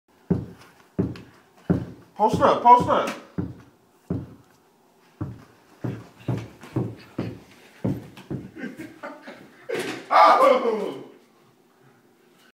CRAZY CROSSOVER LEBRON
Quick crossover on indoor court